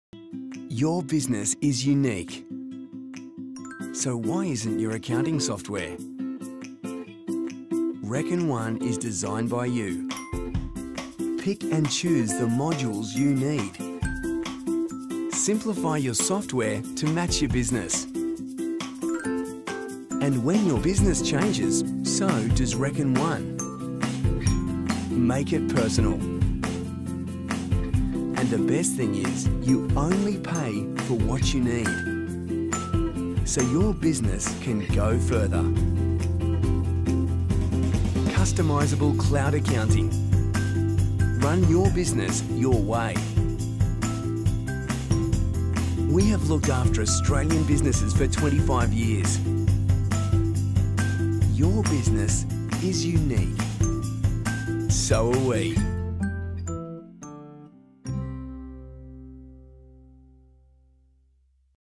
Reckon One is Now Widely Available For Australian Small Business!
It's here! Reckon One, our new online accounting solution is now widely available for Australian small businesses! You can try it out for free.